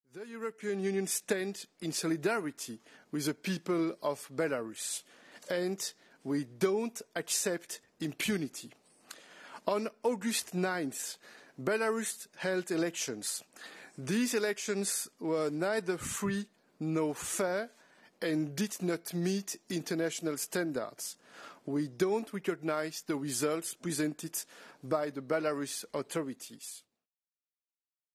EU does not recognise Belarus election result | AFP
The European Union does not recognise the outcome of the presidential election in Belarus says European Commission President Charles Michel, adding that the controversial vote was "neither free nor fair".